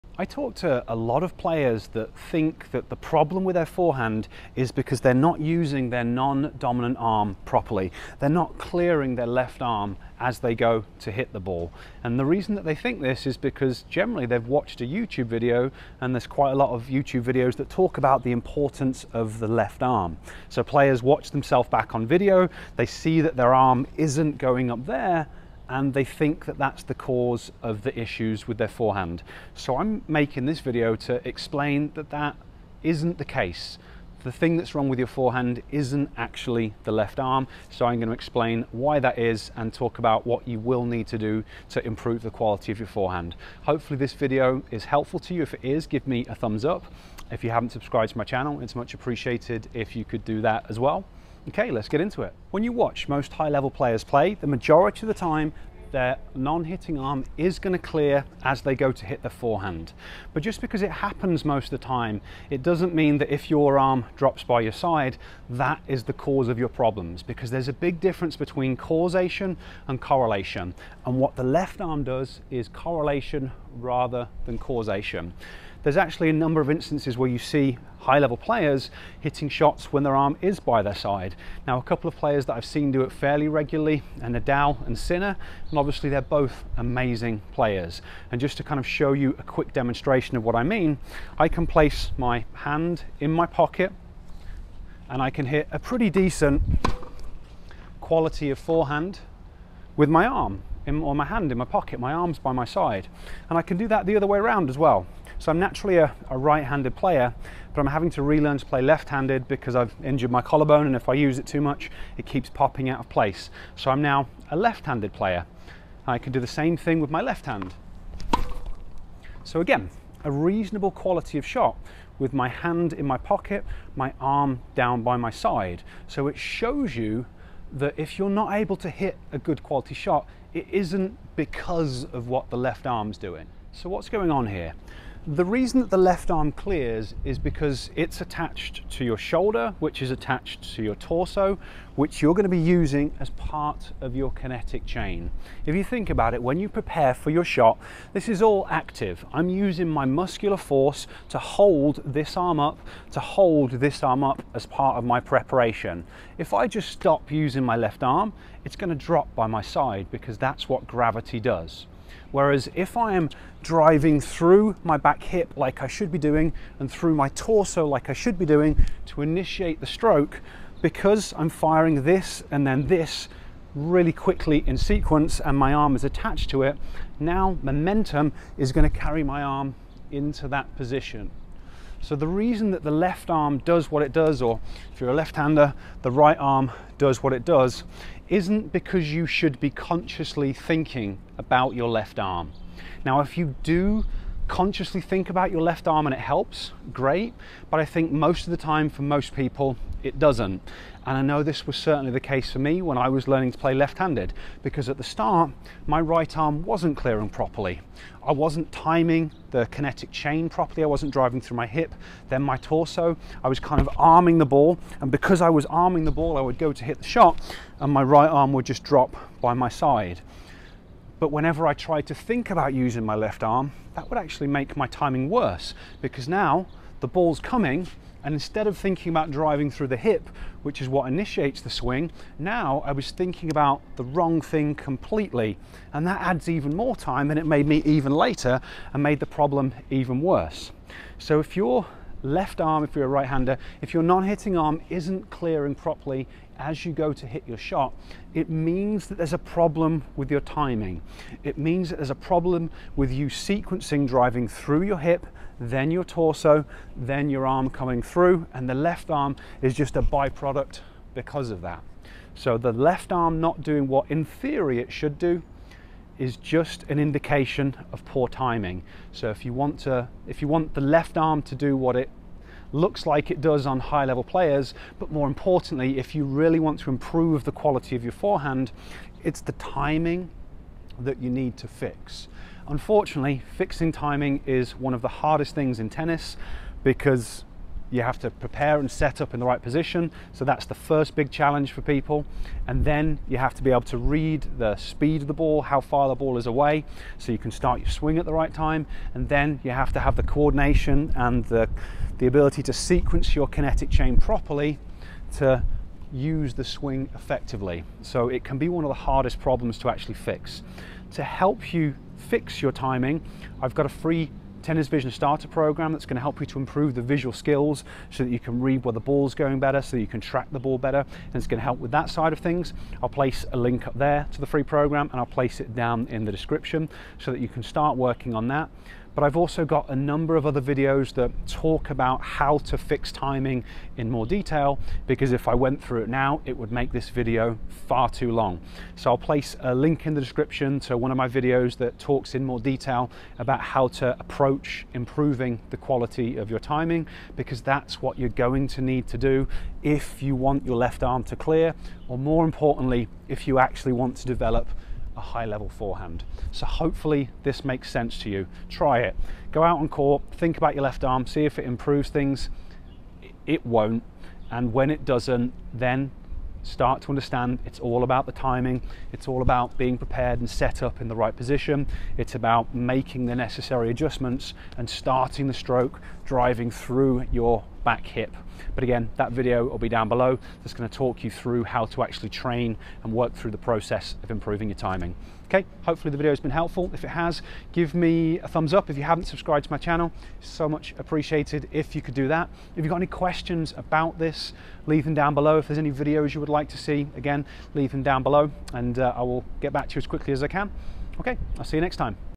Is your non dominant arm ruining your forehand?
There are a lot of coaches that talk about the importance of the non-dominant arm on the forehand. In this video I explain why it’s never the underlying cause of forehand problems. I also talk about what you need to do if you want your arm to clear, like you see from so many high level players.

RESOURCES MENTIONED IN VIDEO